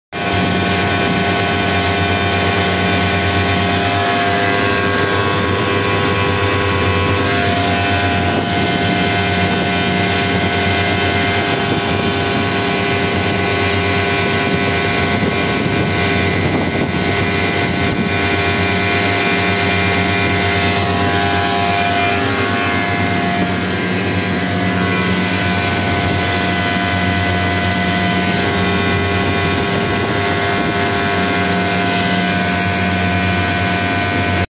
Discharge line leak Danfoss VZH170 VFD scroll compressor
Danfoss VZH170 VFD Scroll compressor had a small leak in the discharge line. the leak is between the pipe and the discharge female fitting. the compressor was operating at full load (200 Hz), and unfortunately, the compressor lost all its oil and the rotor was locked afterward,